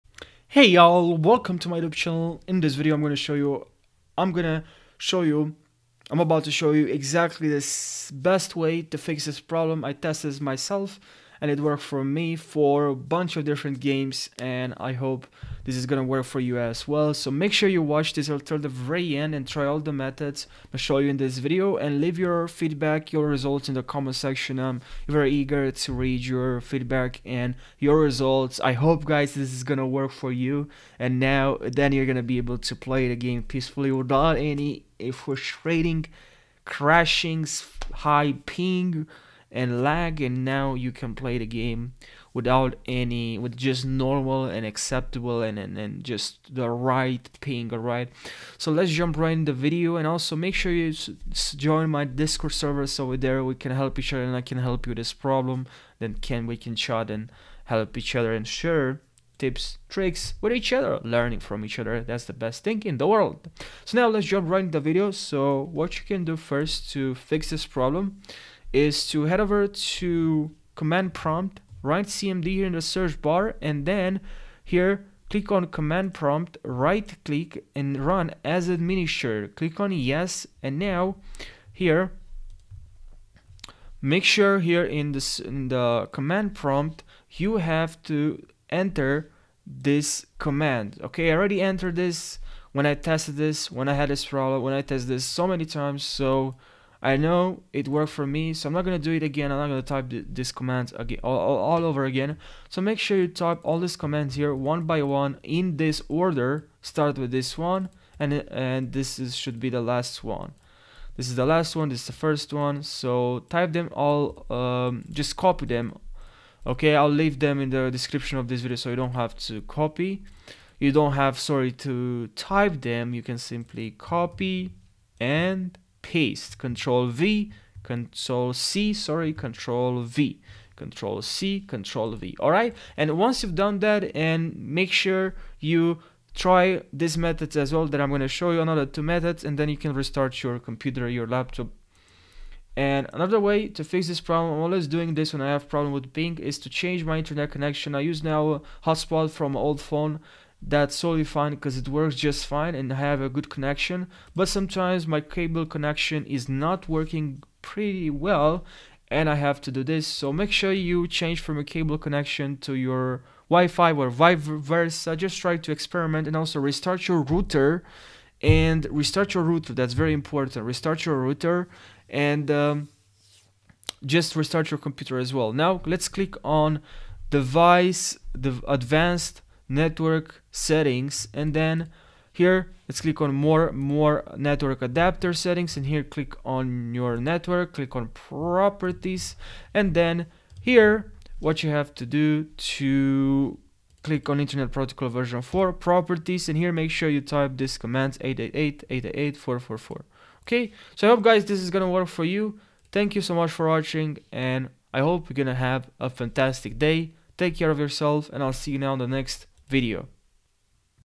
🛠️ Rainbow Six Siege High Ping | Fix This Issue With This Methods - Full Tutorial
You don't wanna miss any part of this tutorial 'cause I'll walk you through every step. So, make sure to stick with me and watch the whole video about how to fix high ling in rainbow six siege fast

⌚ TIMESTAMPS
0:10 Introduction
03:20 Wrap up